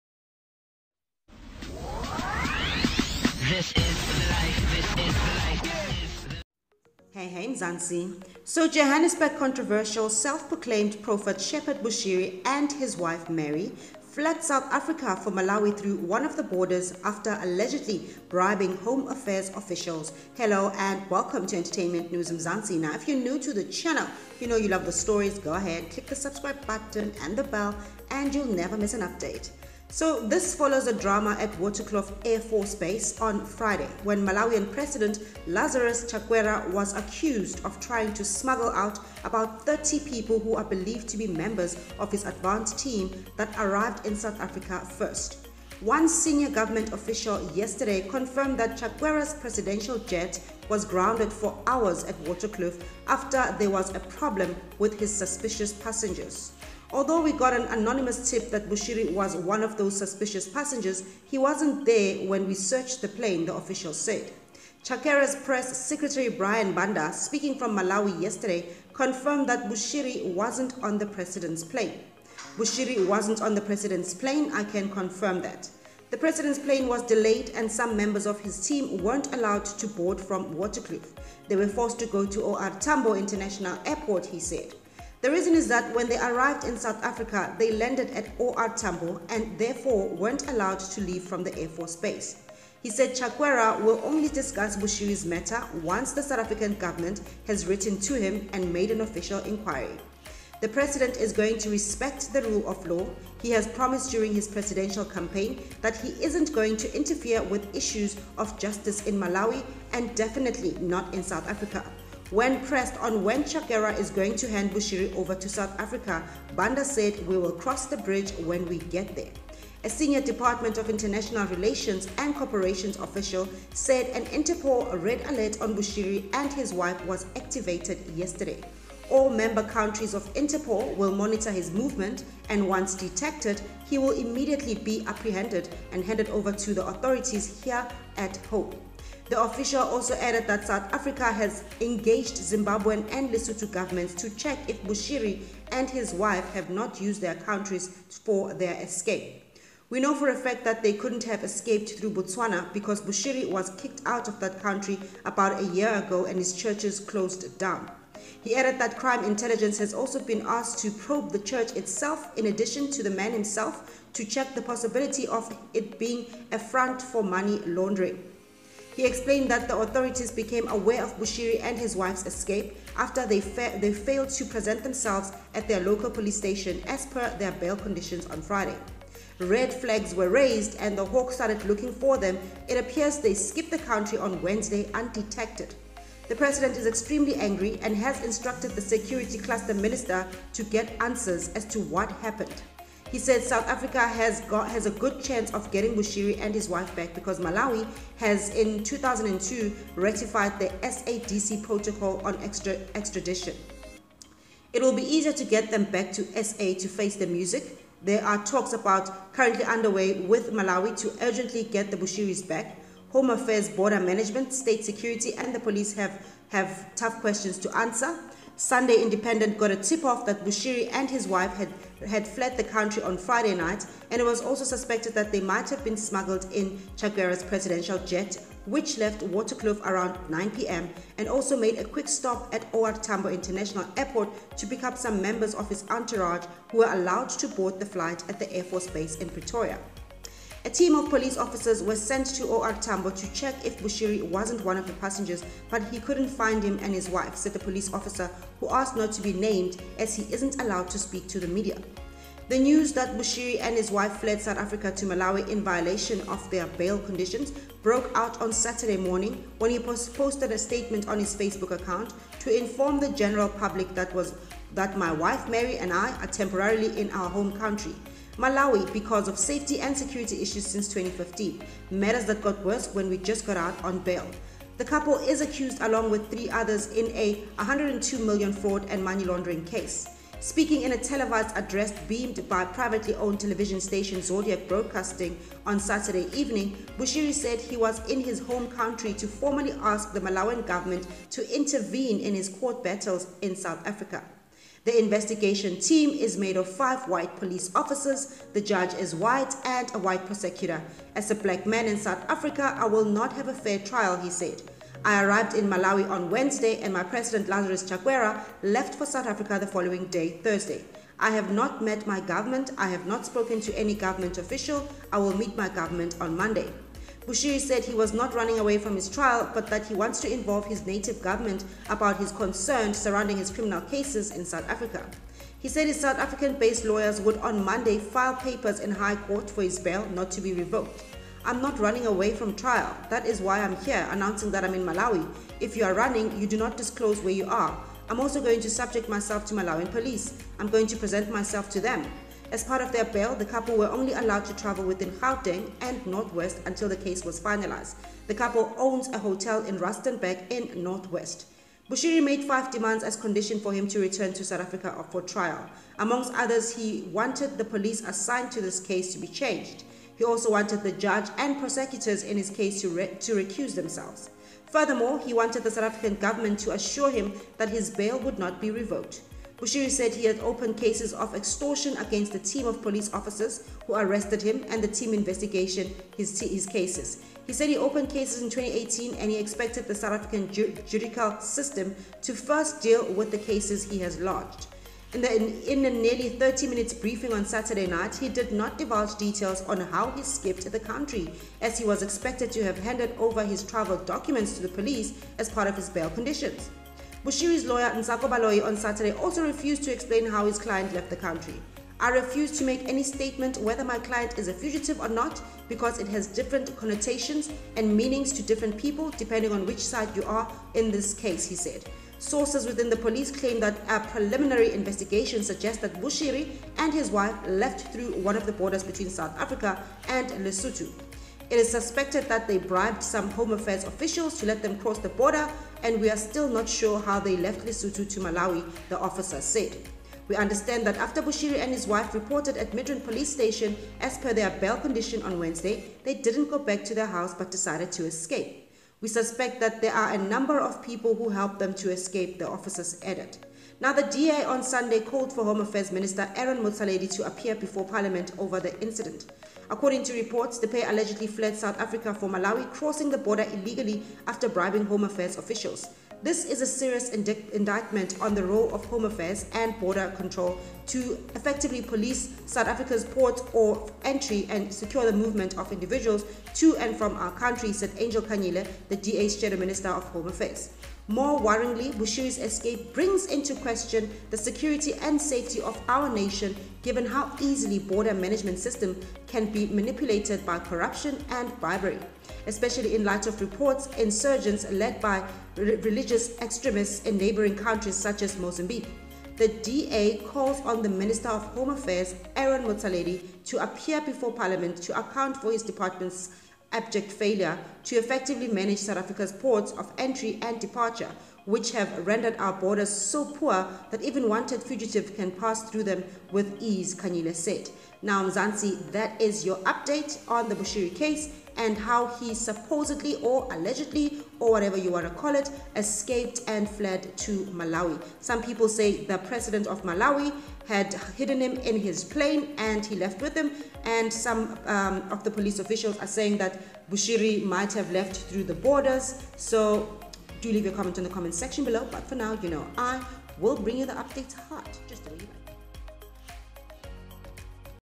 How did Bushiri pull off the great escape, was Malawian President involved?
How did Bushiris pull off the great escape, was Malawian President involved?